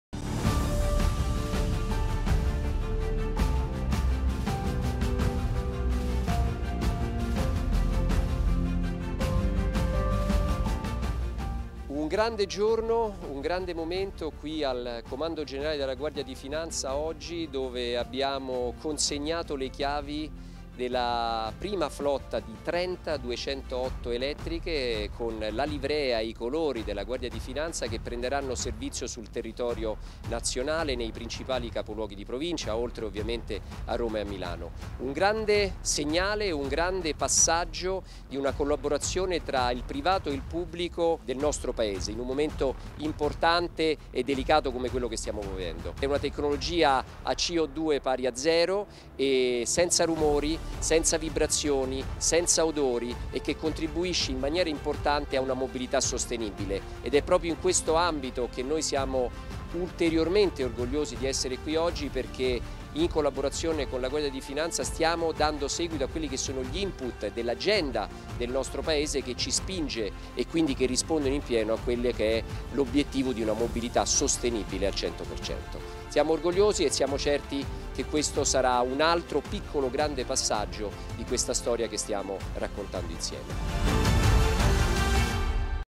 Consegnata la flotta di Peugeot e-208 alla Guardia di Finanza - Electric Motor News n° 24 (2021)
Nel Comando Generale del Corpo della Guardia di Finanza è avvenuta la cerimonia di consegna della flotta di 30 Peugeot e-208, con la presenza delle più alte cariche istituzionali. Le auto, tutte in livrea d’ordinanza, sono state fornite con contratto di noleggio tramite la società Leasys (brand del Gruppo Stellantis) e saranno destinate a diverse città del Paese.
Nel video, la parola di Salvatore Internullo, Direttore Generale Peugeot Italia.